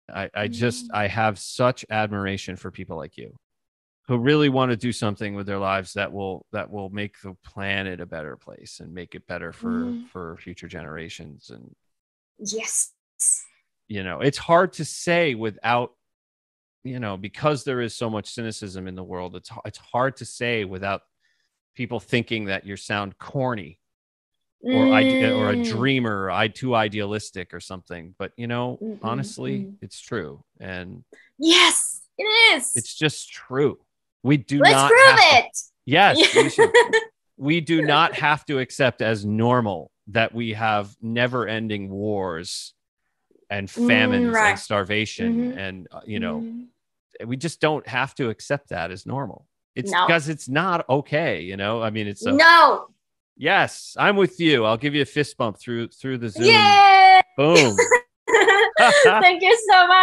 World Peace! My guest from episode 7, Shihori and I share our common commitment to world peace.
In episode 7 with my guest, singer, songwriter, J-pop star and Anime music sensation, Shihori and I discuss her life and career in music, and her commitment to creating art that will make the world a better place. ❤️